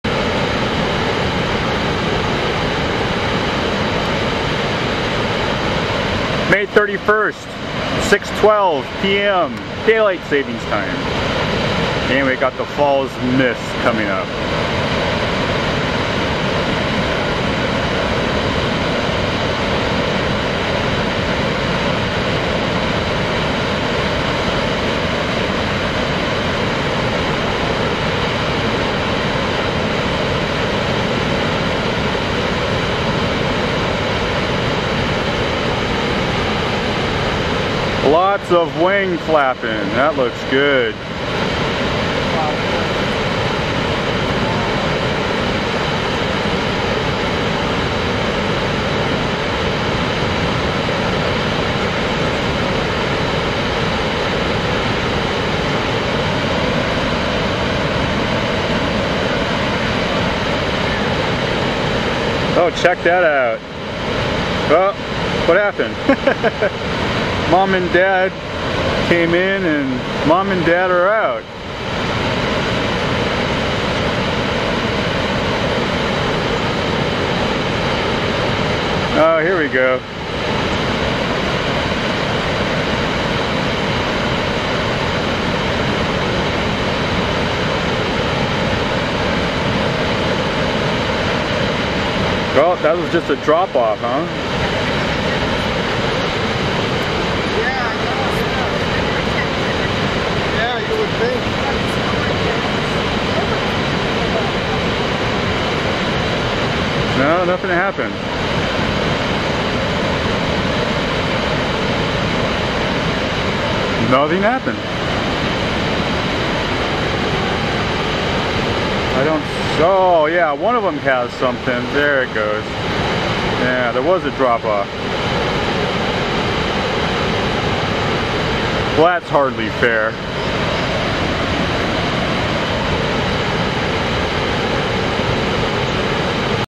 May 31 2012 Peregrine Falcon feeding at Snoqualmie Falls
Both the adult male and female fly into the nest, but the food slips off the ledge, not to worry, it's grab on the way down, and brought back into the nest, but, only one gets the best of the meal.